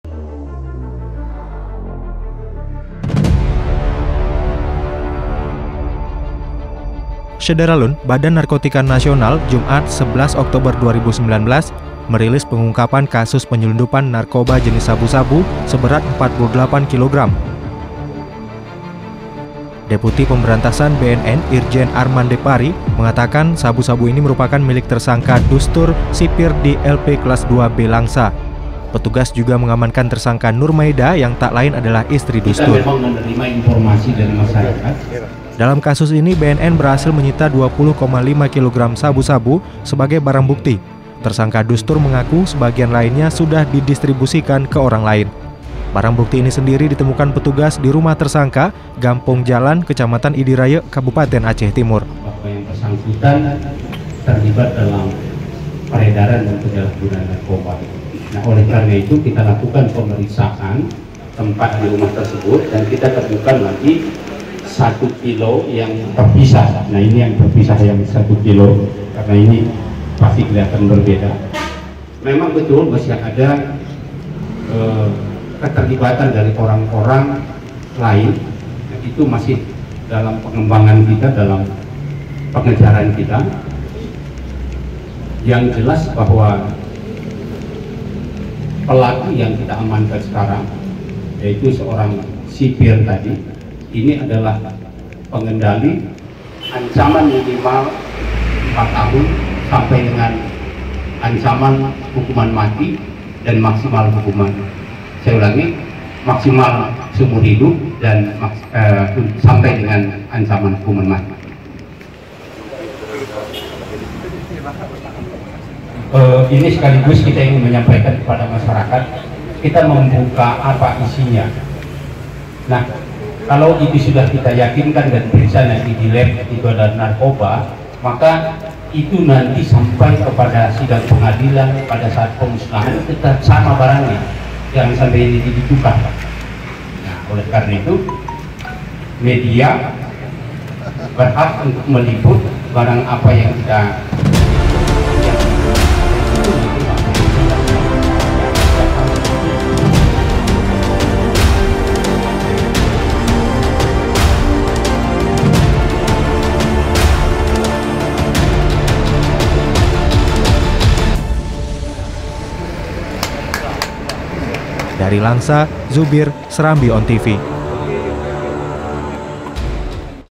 BNN Pusat Ungkap Kasus Sabu 48 Kilogram yang Melibatkan Sipir LP di Langsa
SERAMBITV.COM, LANGSA - Badan Narkotika Nasional, Jumat (11/10/2019) merilis pengungkapan kasus penyeludupan narkoba jenis sabu-sabu seberat 48 Kilogram.

Deputi Pemberantasan BNN, Irjen Arman Depari, sabu-sabu ini merupakan milik tersangka Dusthur, Sipir di LP Kelas II B Langsa.

Petugas juga mengamankan tersangka Nur Maida, yang tak lain adalah istri Dusthur.

Dalam kasus ini, BNN berhasil menyita 20,5 Kilogram sabu-sabu sebagai barang bukti. Tersangka Dusthur mengaku sebagian lainnya sudah didistribusikan ke orang lain.

Barang bukti ditemukan petugas di rumah tersangka, Gampong Jalan, Kecamatan Idi Rayeuk, Kabupaten Aceh Timur.

Penangkapan tersangka berlangsung di lokasi terpisah, dimana Dusthur ditangkap di Kota Langsa, sementara isterinya diamankan petugas dari rumahnya.